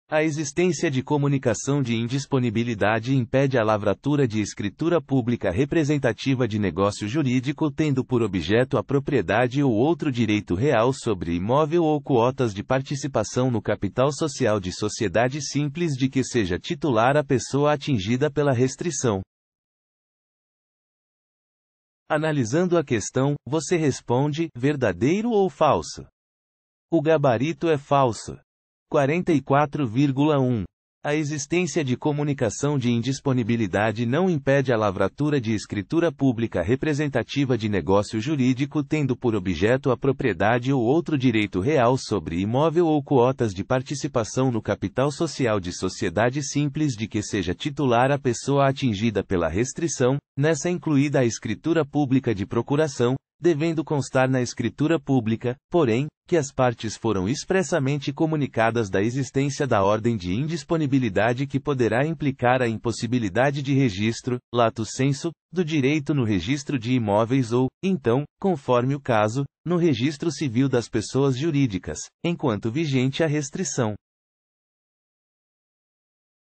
QScon Direito: [Item 44.1, Cap. XVI, NSCGJSP] Decorando a Lei Seca
Item 44.1, Cap. XVI, NSCGJSP | Decorando a Lei Seca

A existência de comunicação de indisponibilidade impede a lavratura de escritura. pública representativa de negócio jurídico tendo por objeto a propriedade ou. outro direito real sobre imóvel ou quotas de participação no capital social de. sociedade simples de que seja titular a pessoa atingida pela restrição.

Gabarito: Falso